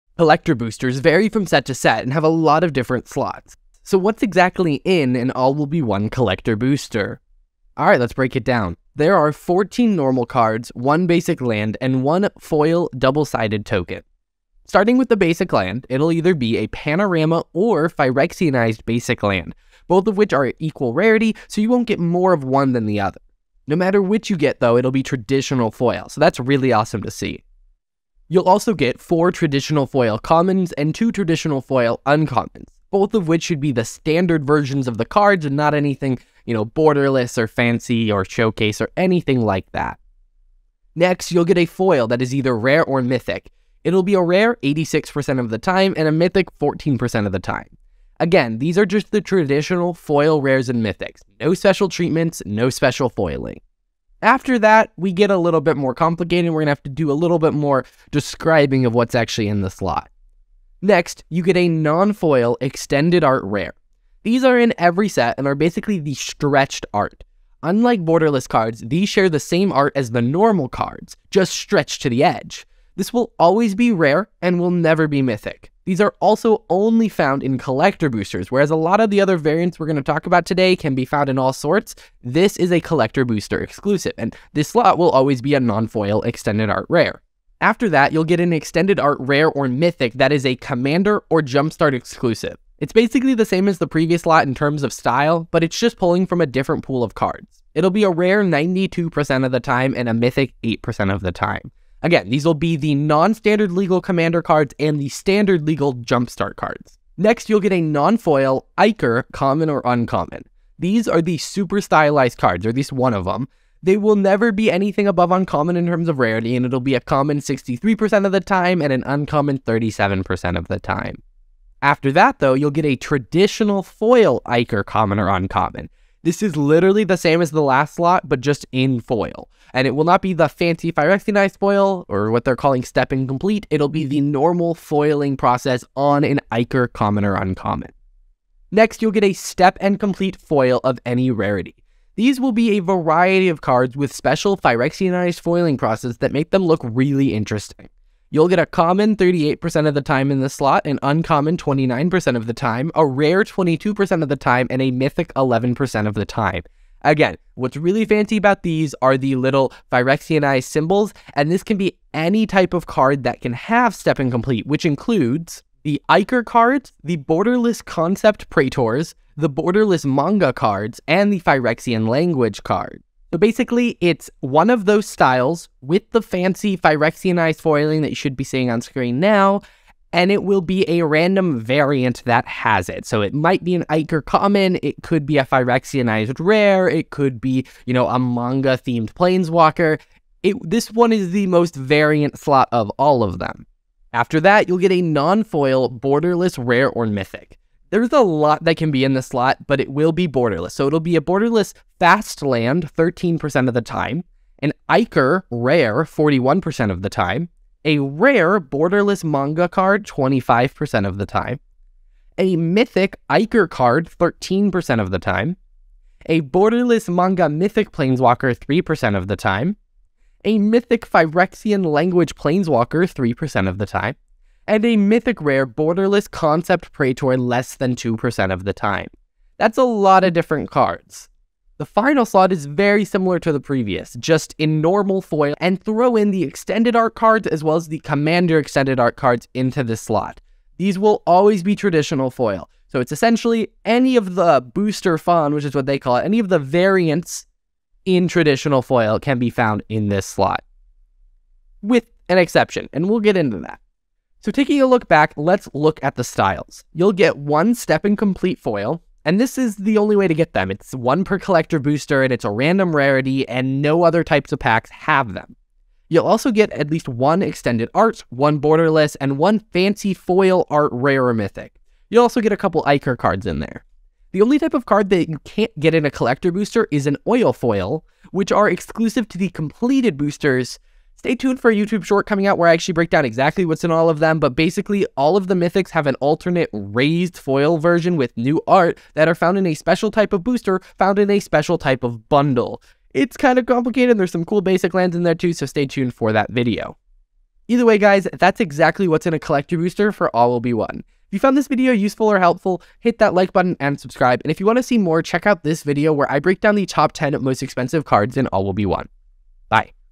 What's Actually In An All Will Be One Collector Booster?
Discover the hidden treasures inside the Magic the Gathering Phyrexia All Will Be One Collector Booster. From rare and powerful cards, to unique tokens and foils, this video is a must-see for any MTG fan. Find out what makes the All Will Be One Booster a must-have addition to your collection. Watch now!